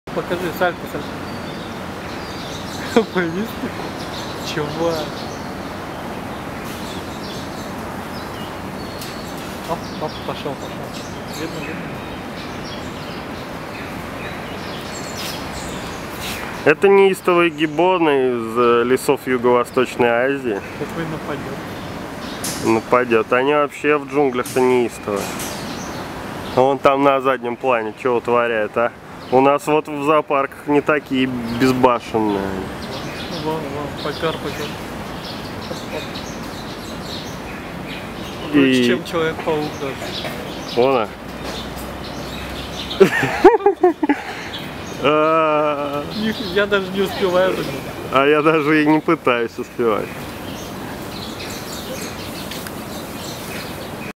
Frenetic Gibbon At Hong Kong Zoological Garden (part1) / Неистовый Гиббон из Зоосада (часть1)
Very nimble and hyperactive monkey.
Очень ловкая, шустрая и гиперактивная обезьяна